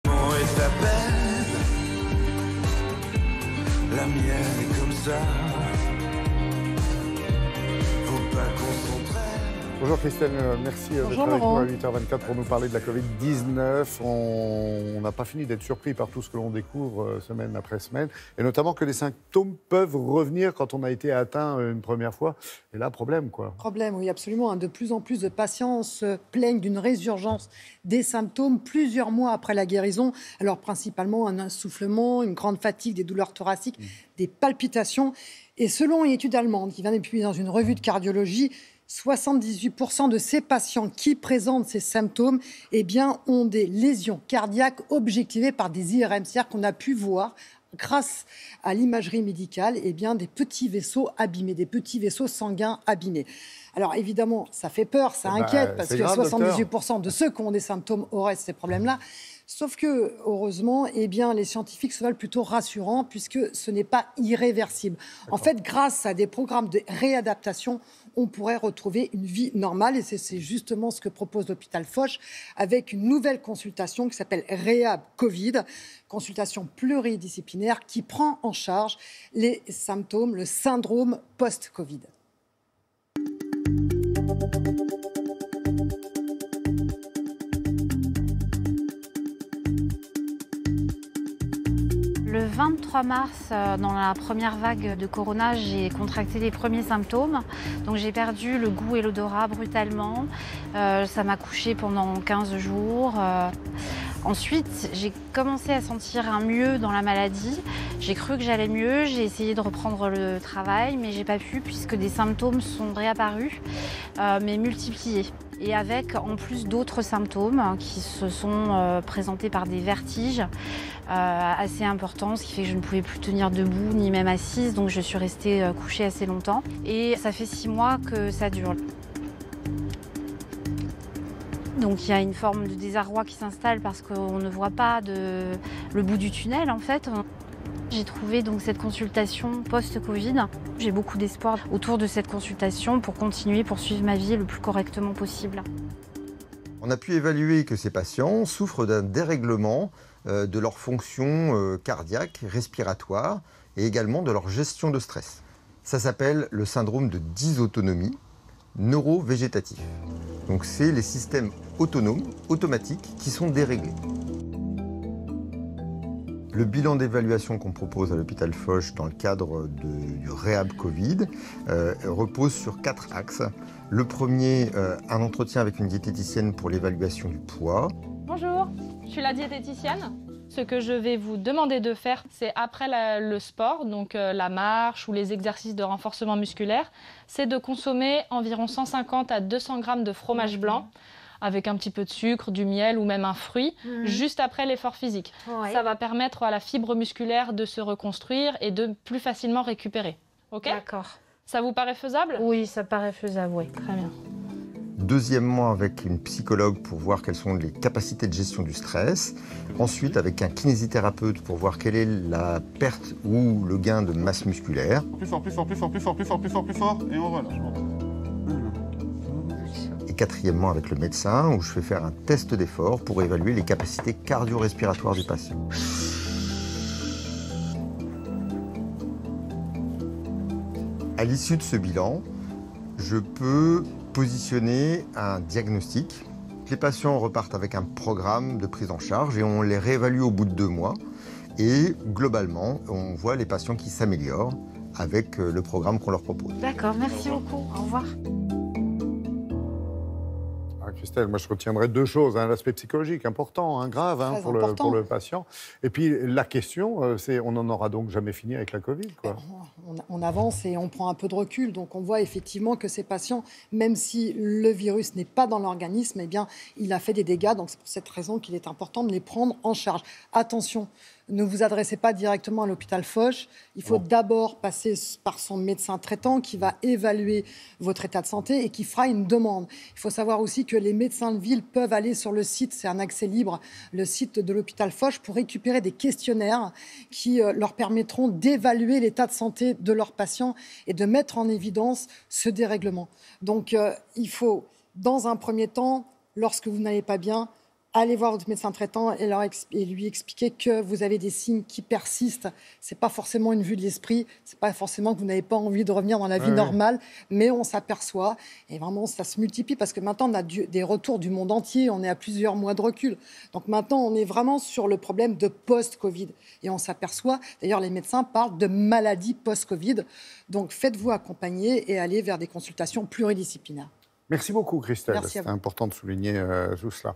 Santé - Réadaptation post-covid
Chroniqueuse : Christelle Ballestrero

De plus en plus de patients de plaignent d'une résurgence des symptômes plusieurs mois après la guérison. L'essoufflement, une grande fatigue, douleurs thoraciques ou même des palpitations sont les plus courants.

Selon une récente étude allemande, 78% des personnes qui présentent ces symptômes ont des lésions cardiaques visibles grâce à une IRM.